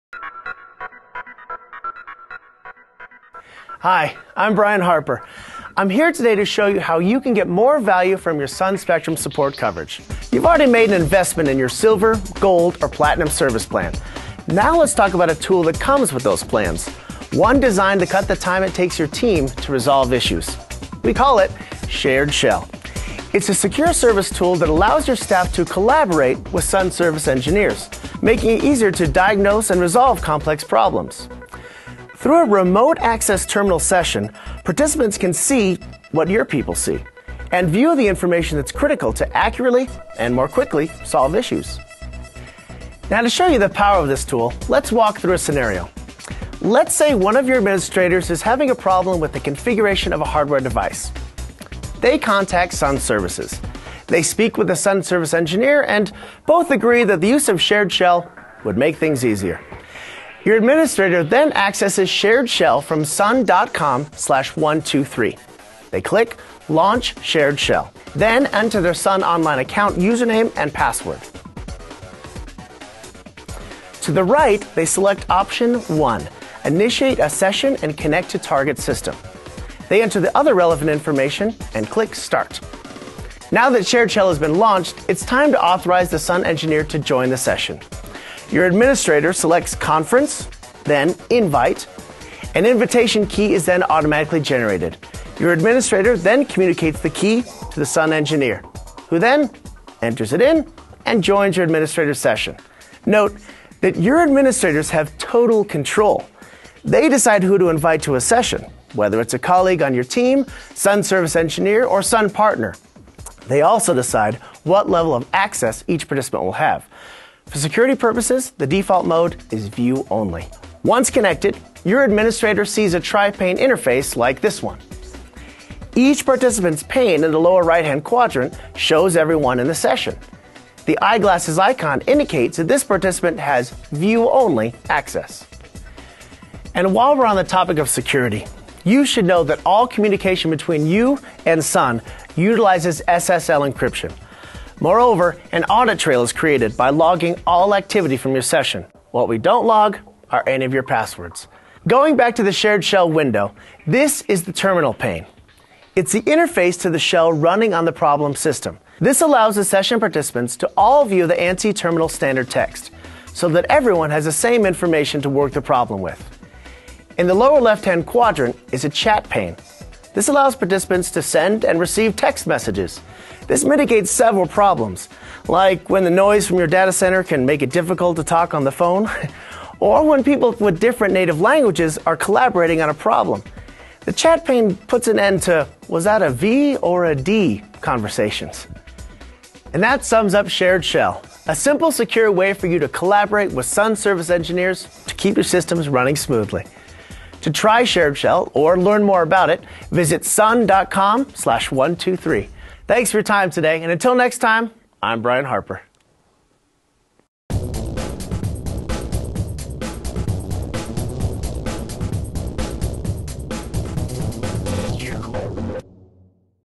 Sun Microsystems Shared Shell
Sun Shared Shell is a collaborative service tool that allows, under your control, Sun Services engineers to remotely view and diagnose your Sun products. It is a Java application that provides secure, shared, remote access to a shell session that both customers and Sun Services engineers view at the same time. You can invite your colleague or additional Sun engineers to participate in the conference as needed. You are always in control of who participates and who has access to the remote access session.